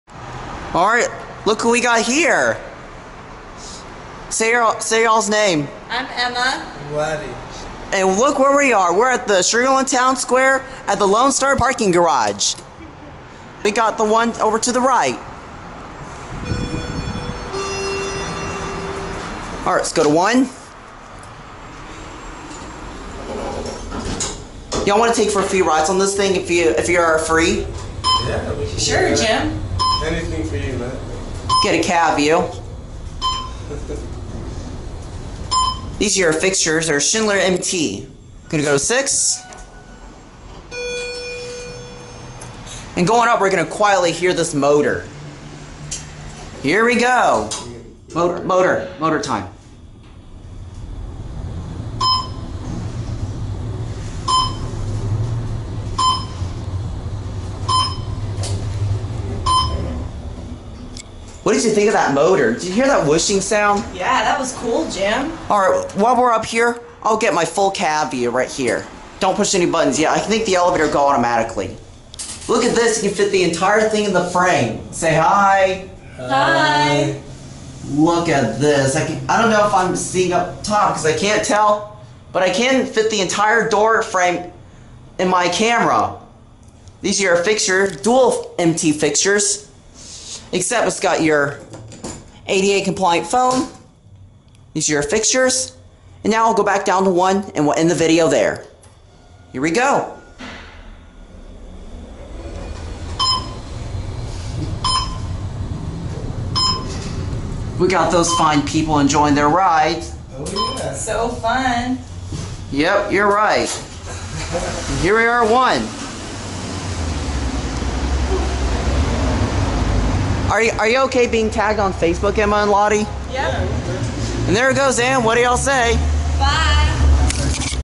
May 2014 Video: Emma & Ladi rides the Schindler 500A traction elevators in Lone Star Garage at SLTS!
Filmed on May 24, 2014: Another TBT video!!! :D These are friends from my high school. Stumbled across them on the top floor while I was riding up and down on the elevators. SLTS stands for Sugar Land Town Square. I understand I have uploaded this 2-3 times in a row, but I (like I said) have a 2017 updated take of both banks of Schindler 500A elevators here in this facility that I will upload later next month or 2. Installed in 2003.